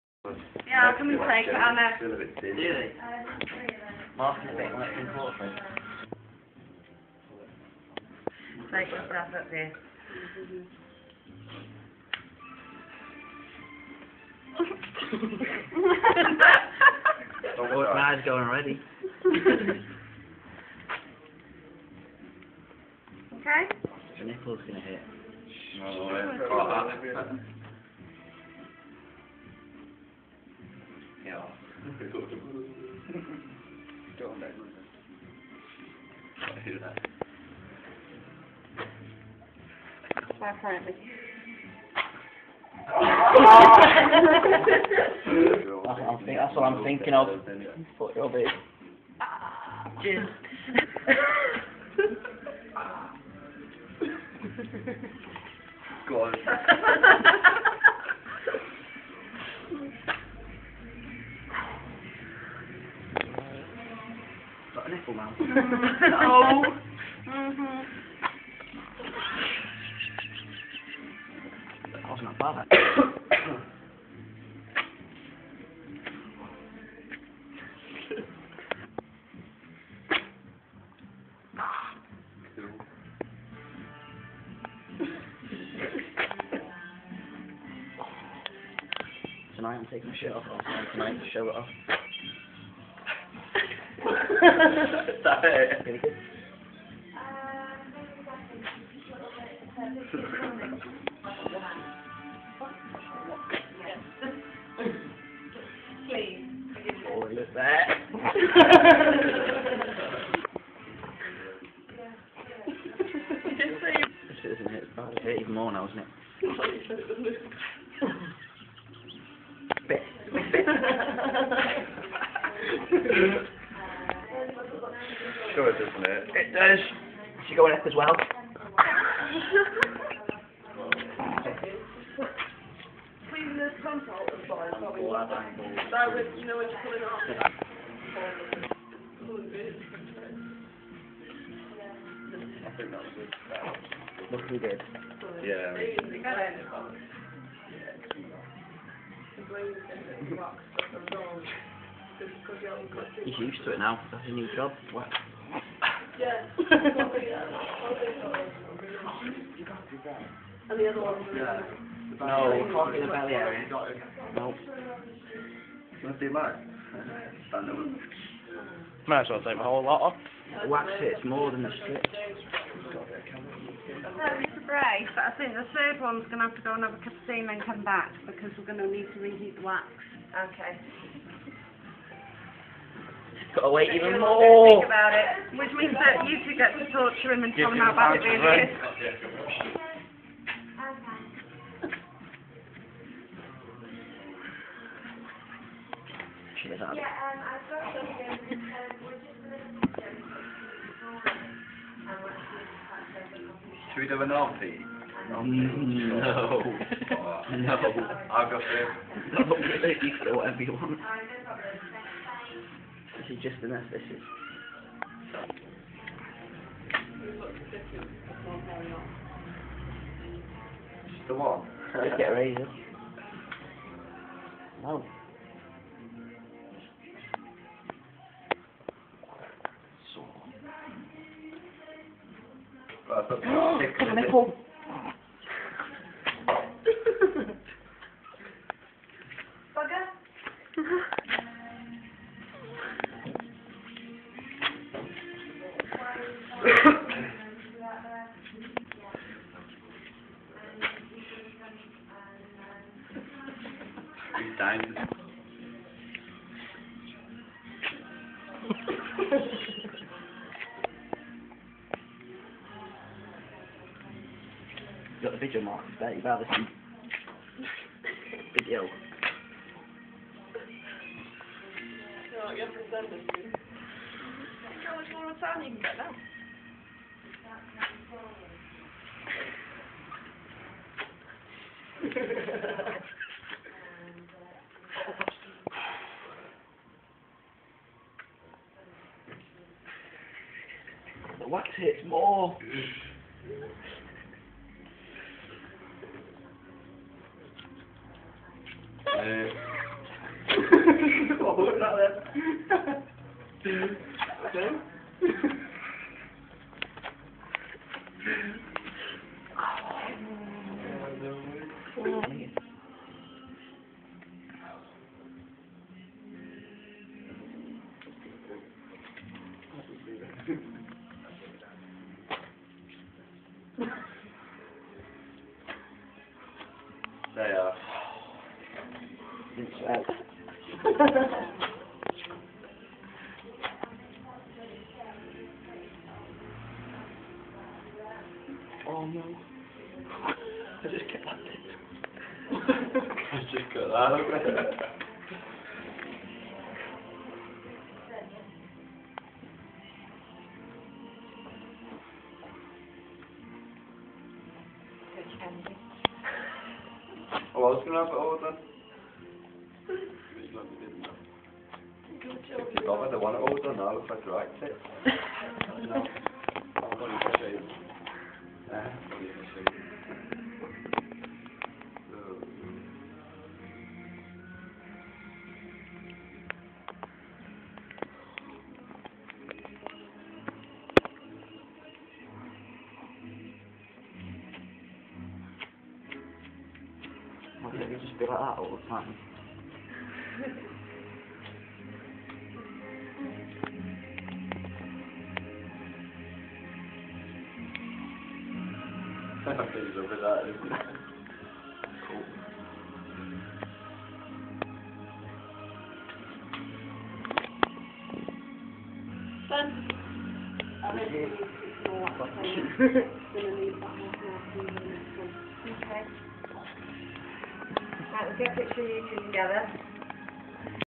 Damien Stiffmister666
Big D getting his Chest waxing at Tone Zone, colwyn bay